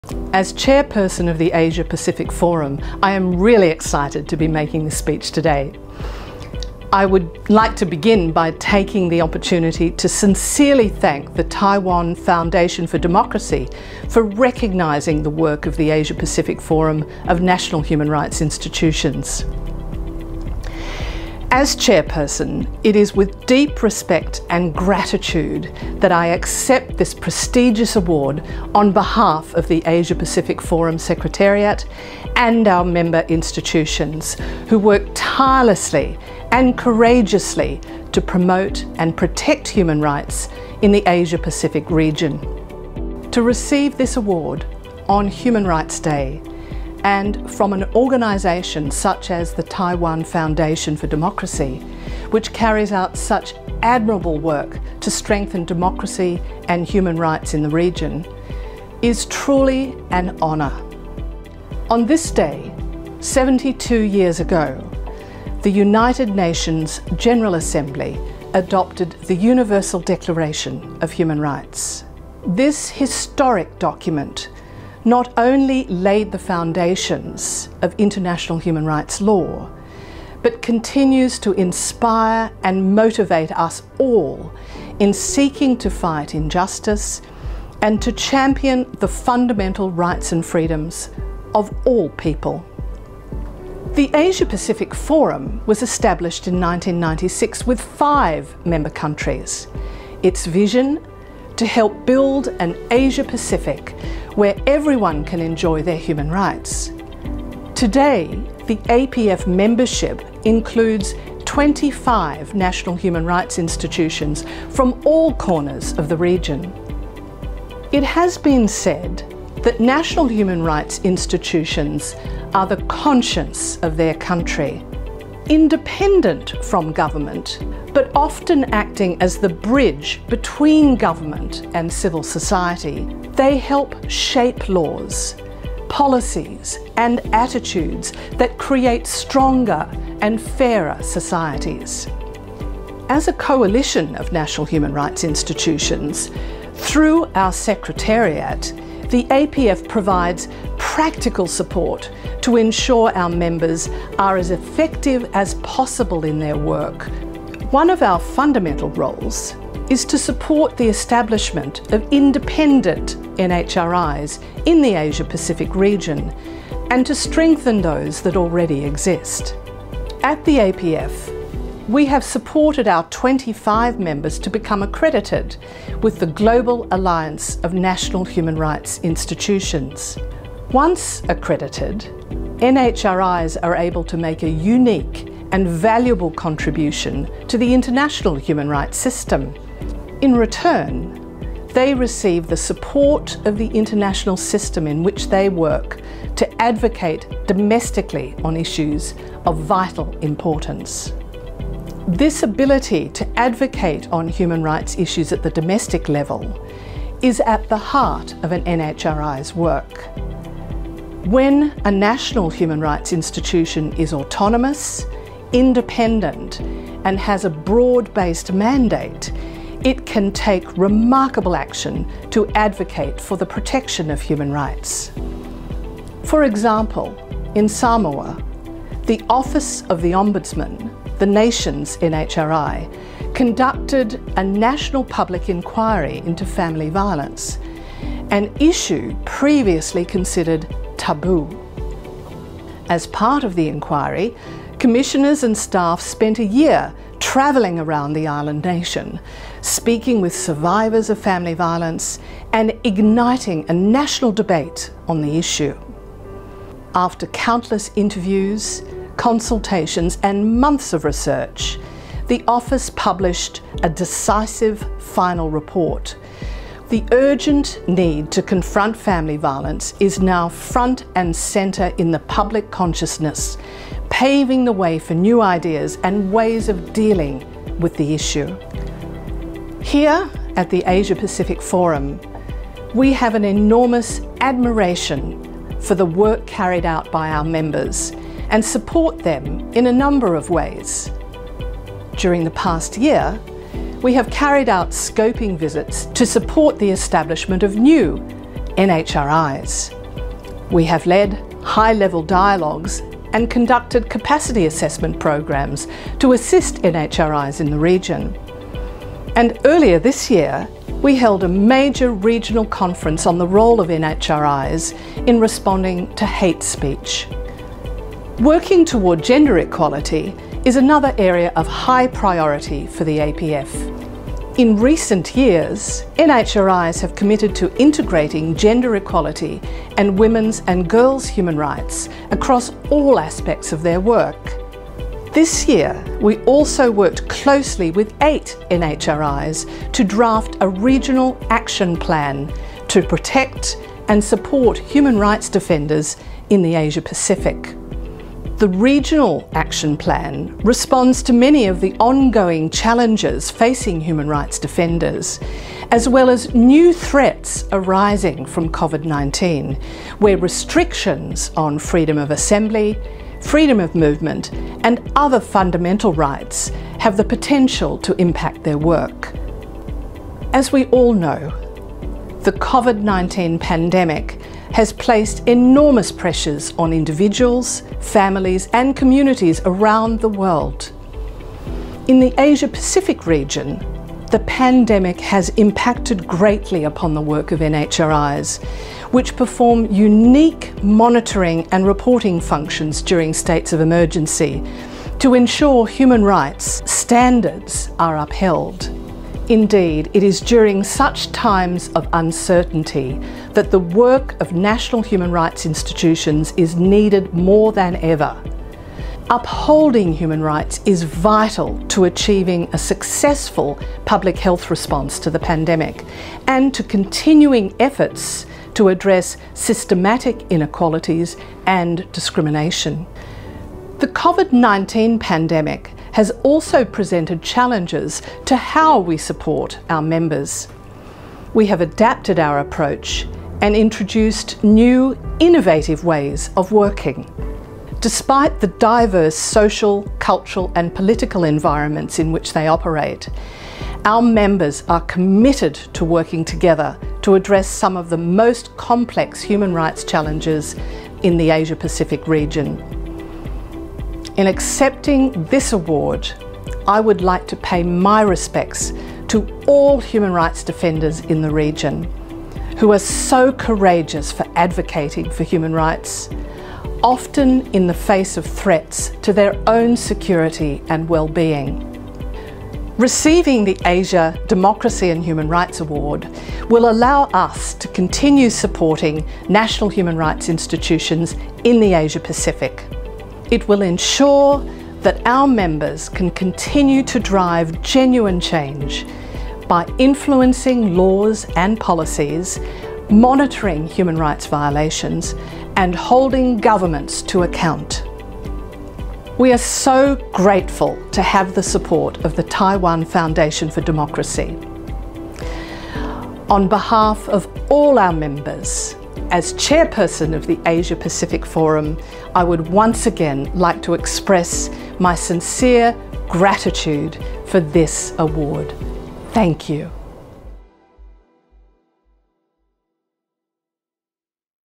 2020 Asia Democracy and Human Rights Award
APF Chairperson Emeritus Professor Rosalind Croucher presents the acceptance speech on behalf of the APF for receiving the 2020 Asia Democracy and Human Rights Award